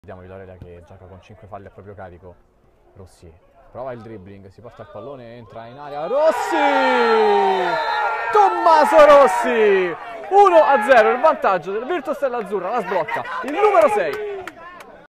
TOP GOL Serie C1 - 8^ | Virtus Stella Azzurra VS Villa Aurelia SC - Rossi (Ste)
Campionato regionale LND Calcio a 5 Serie C1